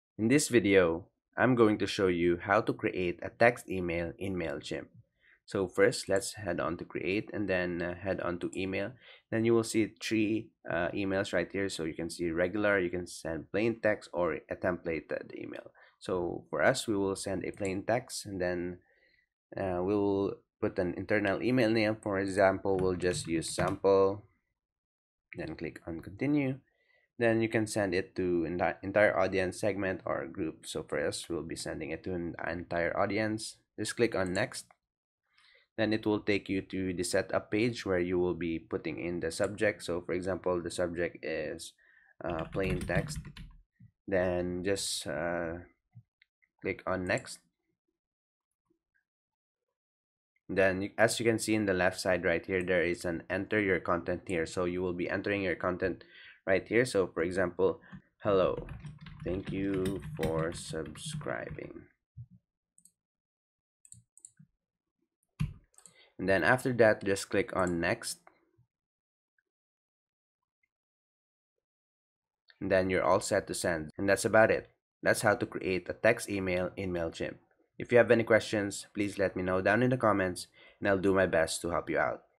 How to Create a Plain Text E-mail in Mailchimp [QUICK GUIDE]
In this video, I'm going to show you how to create a plain text e-mail in Mailchimp.